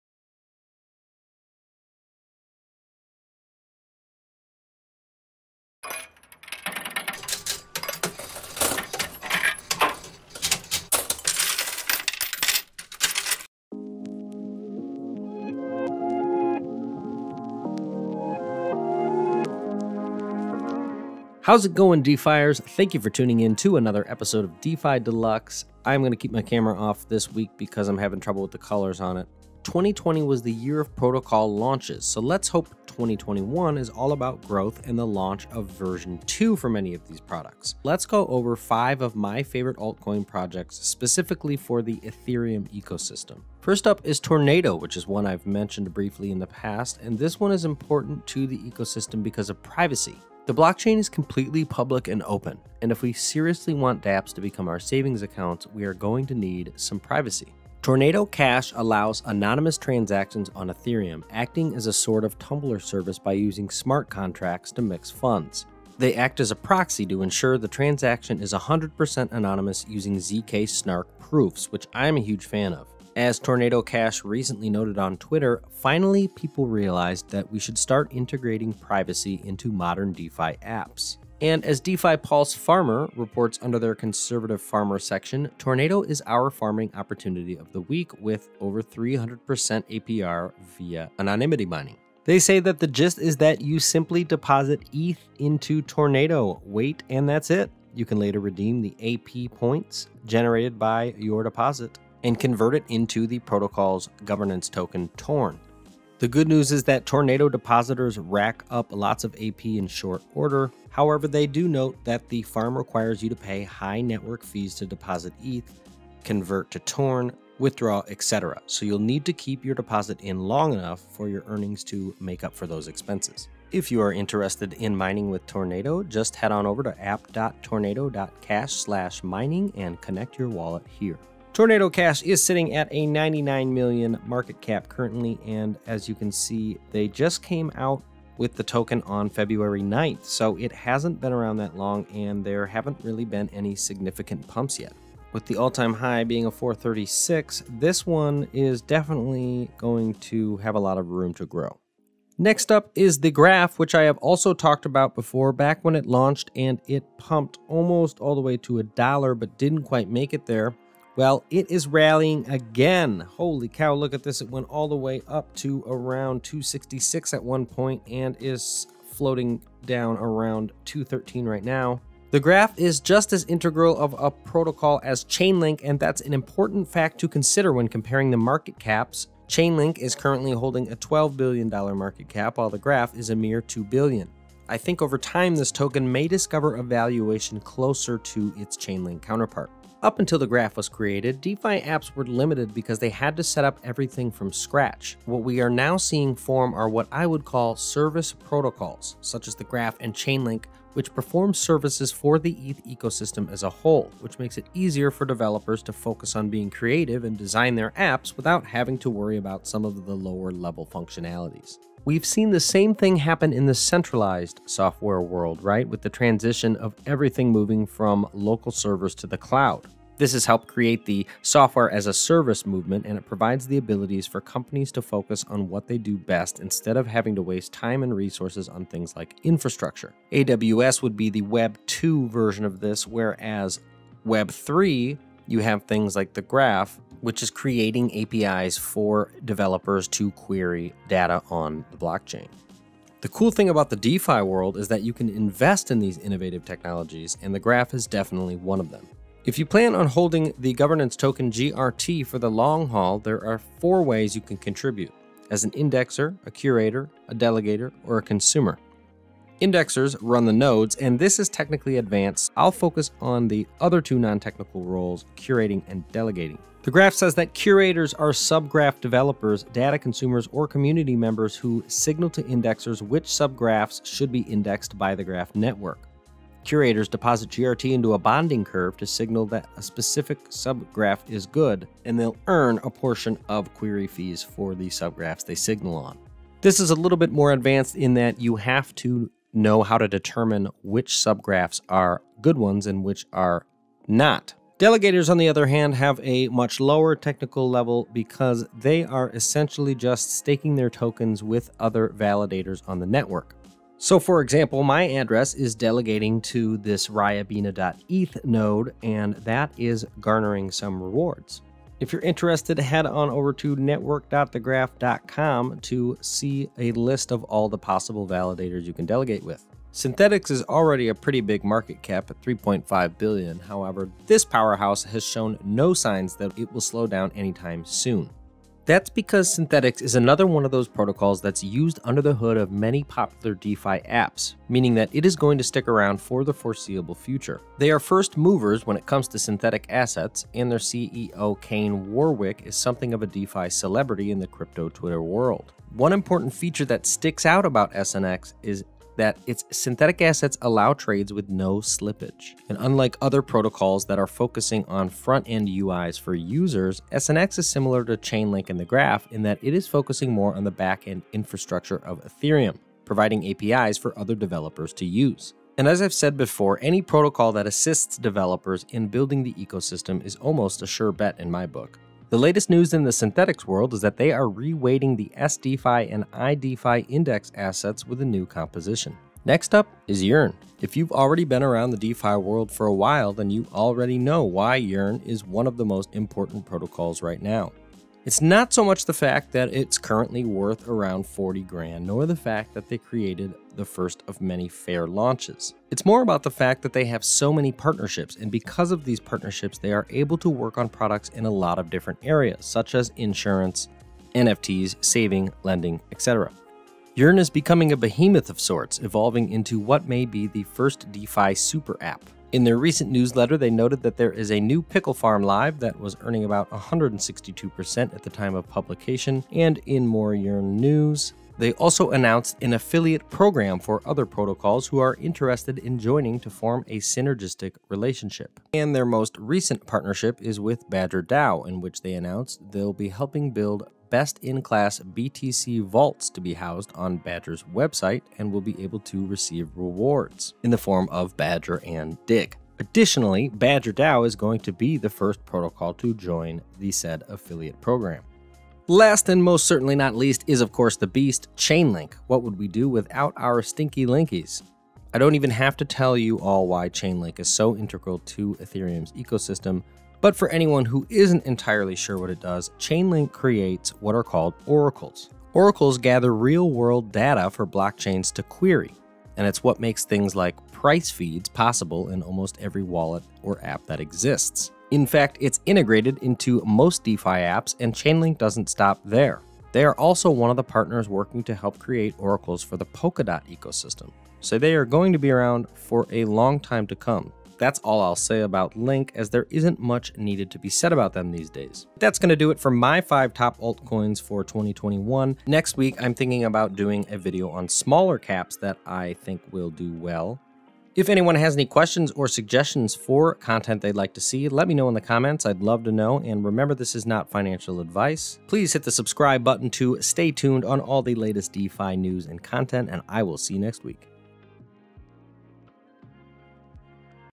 Weekly News in DeFi - My Top 5 Altcoins for 2021! Tornado Mining Incentives! The Graph update + more
Weekly DeFi News Coverage.

Tickers for the above projects are: $TORN, $GRT, $SNX, $YEARN, $LINK

-This technology is experimental and should be treated as such.

ETH or any ERC-20: 0xa857aF9e6CA57B098c5392EED325113D44829665

Daniel Simon, Adobe Stock
Powered by Ethereum

Thanks to boxmining.com for Yearn Ecosystem pic.